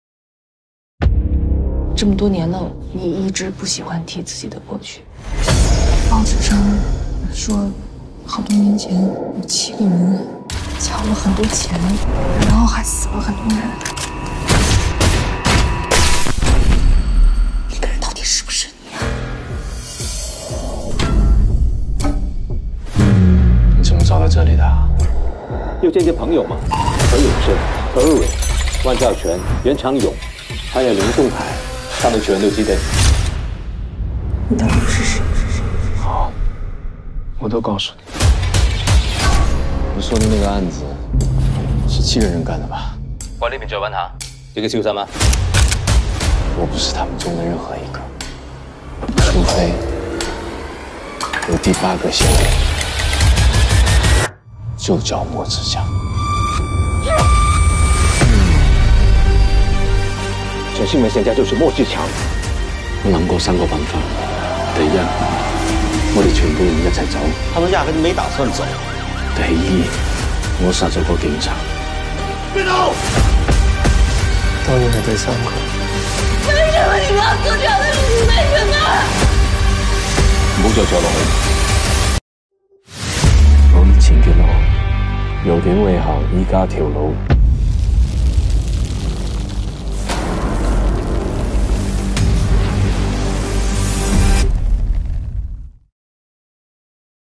DUST TO DUST - A Second Look At This New Gordon Lam & Da Peng Crime Thriller (Hong Kong/China 2023)
Check out Mighty Ape! For the latest in Games, Movies, Toys, Gifts, and other Pop-Culture Items. Very affordable prices and great shipping times:

One of the heists that the movie "Caught In Time" was based on, this thriller looks to be a good time!

Director Jonathan Li
Starring Chengpeng Dong, Gordon Lam, Songwen Zhang
Original Title 第八个嫌疑人
Country of Origin Hong Kong and China
Release Date June 16 2023/September 9 2023

AFF Link